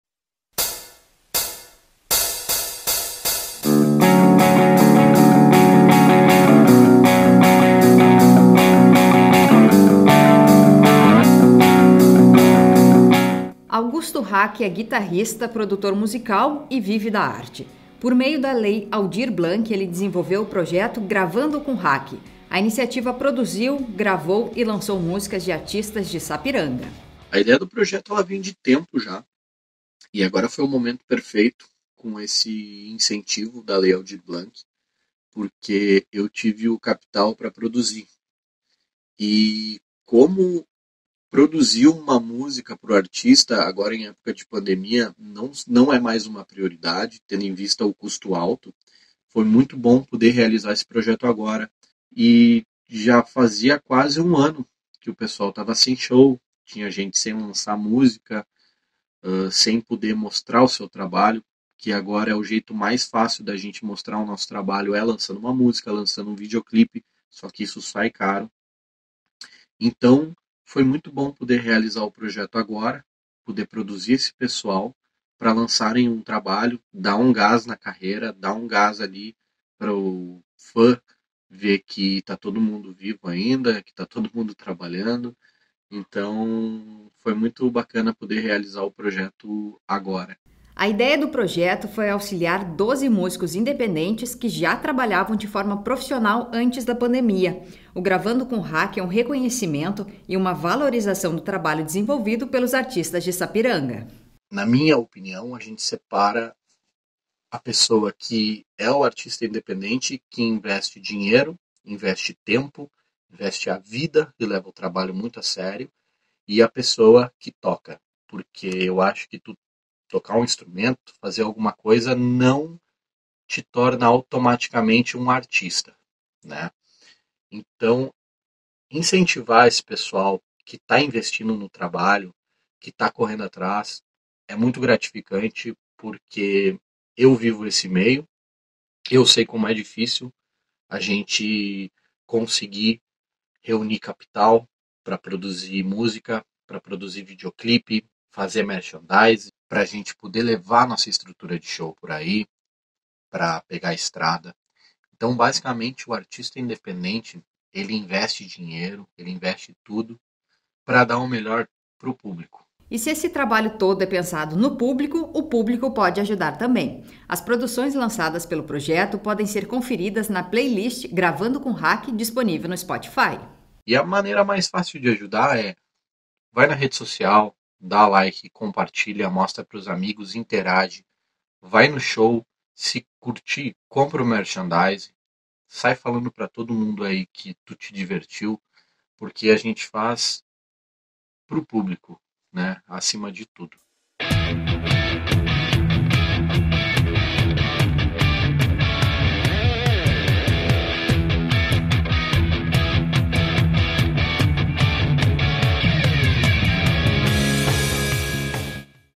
Projeto gravando com Haack
O “Gravando com Haack” é uma iniciativa que incentiva a produção cultural de músicos independentes neste período de pandemia.